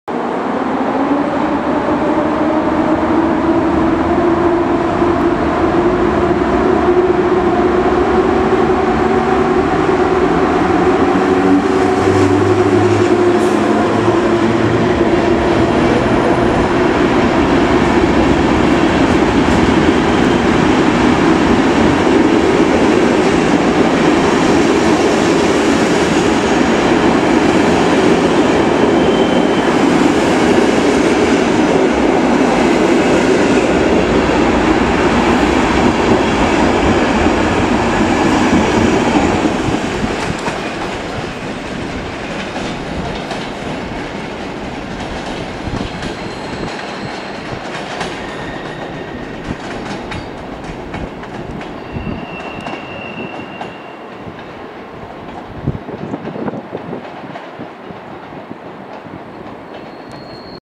Za Darta EU07-303 z IC 16100 Asnyk do Warszawy Wschodniej. 25.04.2018
Data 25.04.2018. Godz. 18:25. Wieczorem na stacji Wrocław Główny. Nagrałem odjazd składu zastępczego za Darta EU07-303 z IC 16100 Asnyk Rel. Jelenia Góra - Warszawa Wschodnia. Zapraszam do oglądania :)